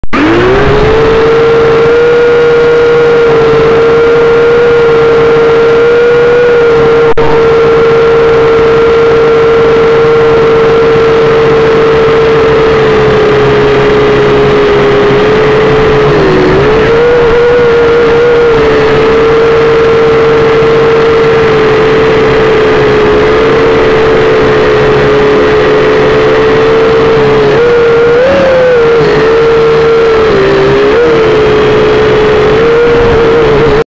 Test Drone Quadcopter H5C Syma
Prova di volo quadricottero H5C Syma. Altezza max circa 20mt.